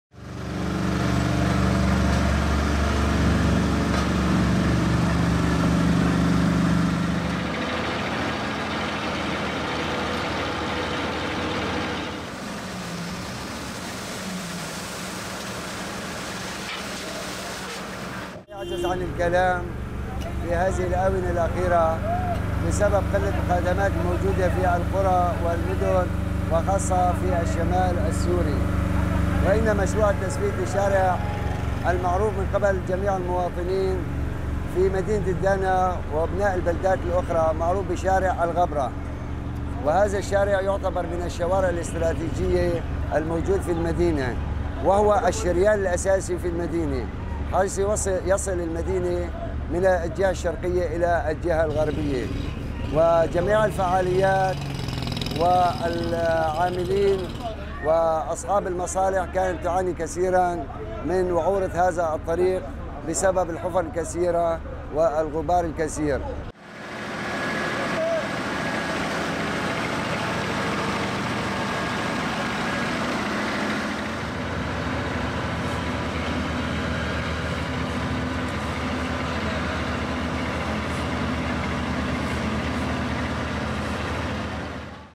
Idlib: Paving Roads In Dana Town 5-7-2017
Several local organizations, in cooperation with the local council of Dana town, started a project for paving the roads in the town, to reuse the roads again.